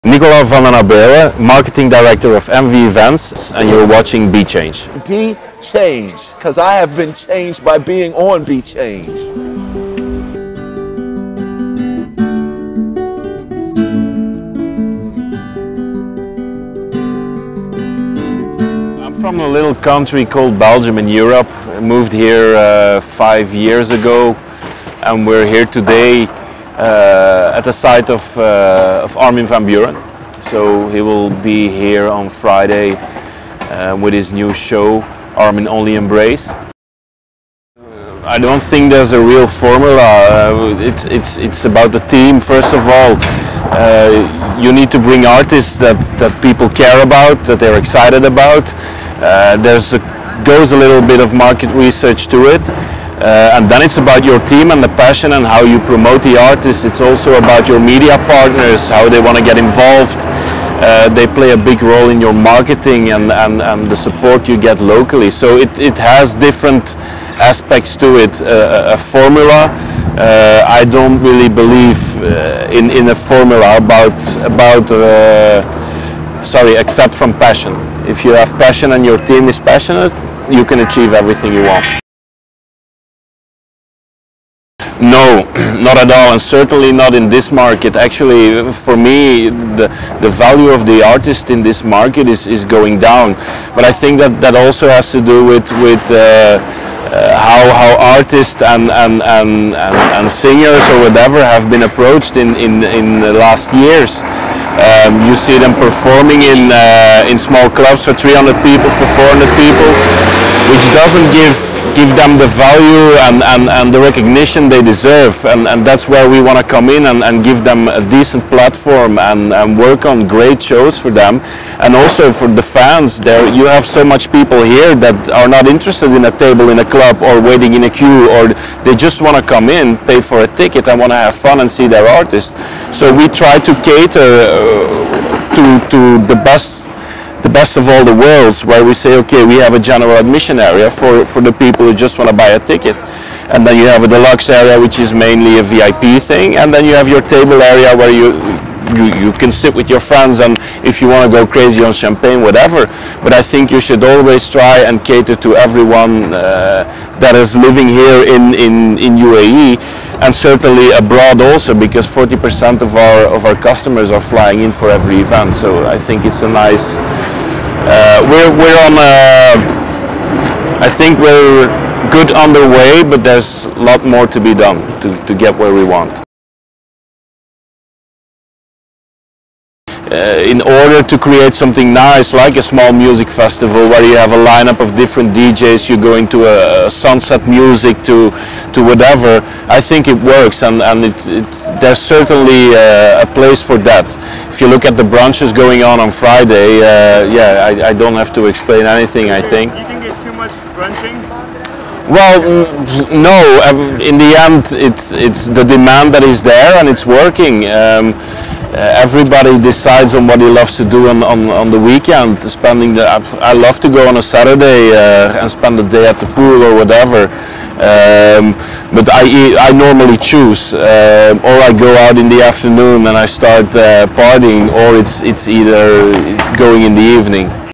Formula for EDM Success in Dubai With Envie Events' Nicolas Vandenabeele
Nicolas Vandenabeele knows a thing or two about staging a successful concert, having hosted the first Sensation White outside the Netherlands at Meydan in Dubai last year. He followed those that with the likes of Martin Garrix and Hardwell live at Meydan as well.

Envie Events in Dubai is now geared up to bring in Armin van Buuren's Armin Only Embrace world tour to Dubai this coming Friday (May 20, 2016).

Ahead of the big gig, Nicolas takes us backstage and talks to us about the clubbing trends in Dubai, quashes artist demand rumours and shares some of the happenings that go behind staging huge events in Dubai.